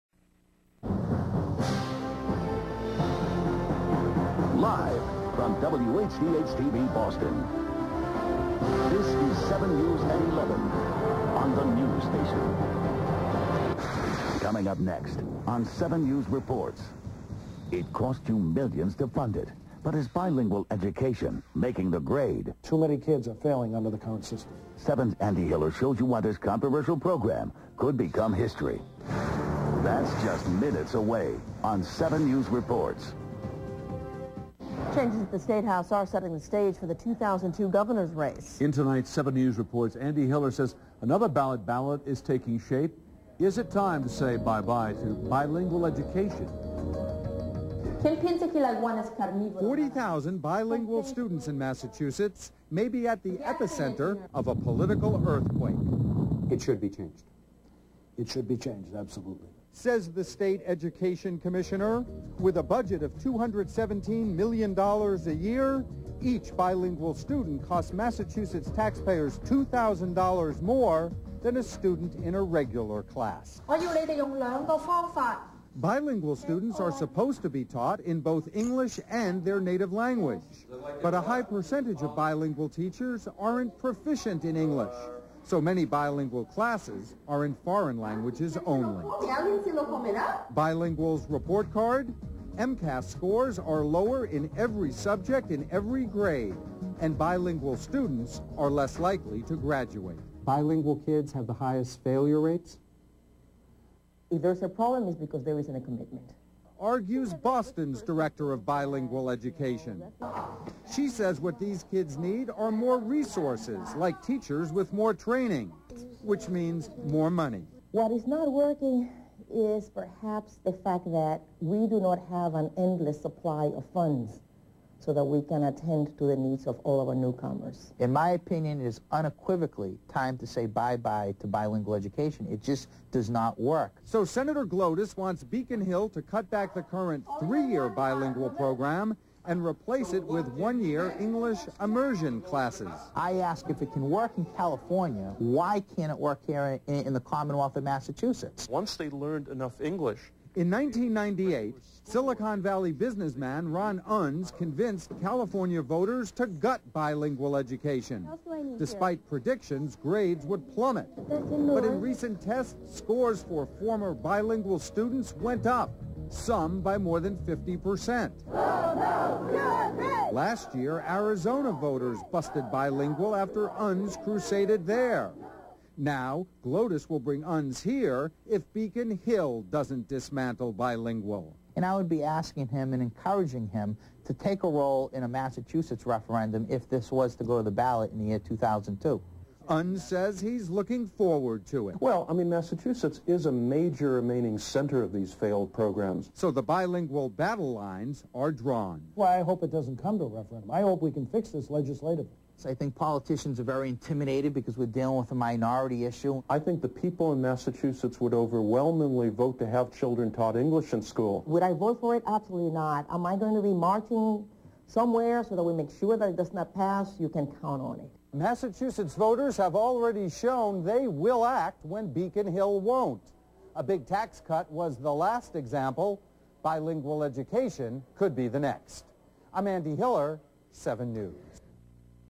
Bilingual Education New England #20 - News, WHDH/Boston, February 20, 2001
Report on bilingual education controversy in Massachusetts, proposed legislation; clips of David Driscoll, Nydia Oldemo-Mendez, Guy Glodis, Ron Unz